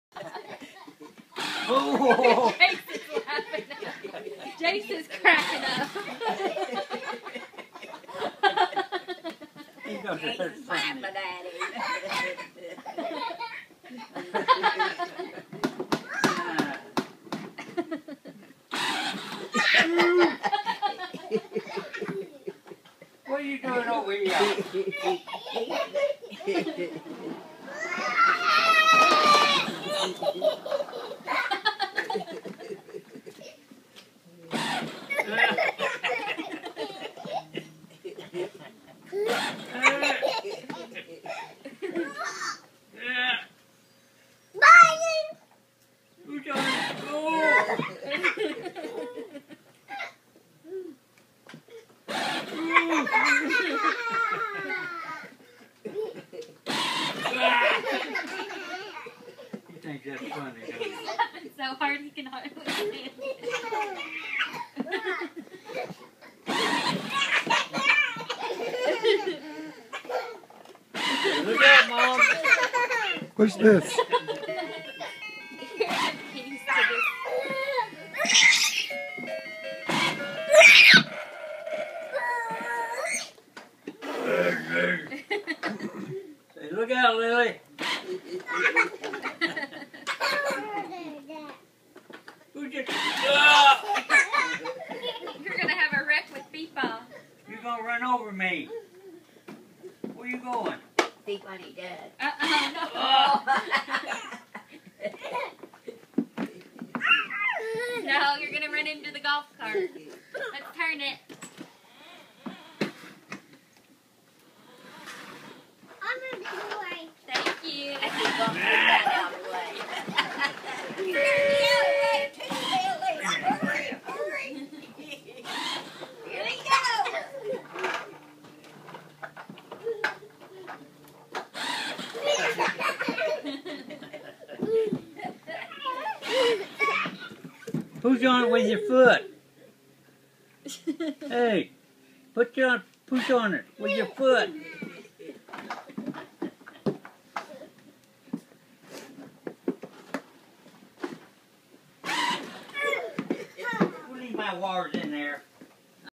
Jackson and his golf cart
Jackson trying out his golf cart at Nana and Peepaws in KY